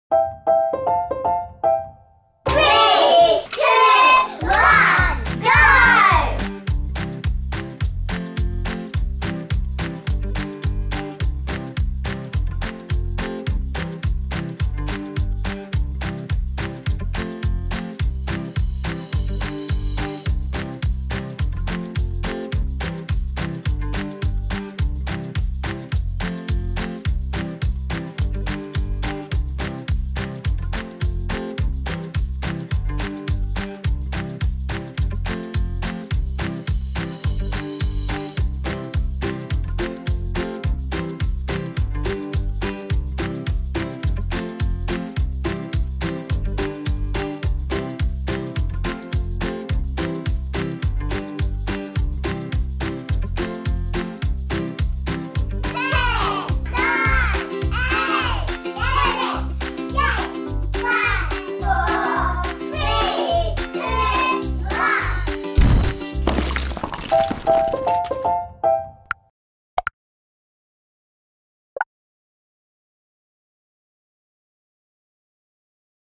1 Minute Blocks Timer - Kids Visual Timer
Watch the blocks build up all the way to the sky! Once the blocks reach the top... TIMES UP! Don't forget to keep an eye out for some surprises along the way.

Sound FX from Zapsplat.com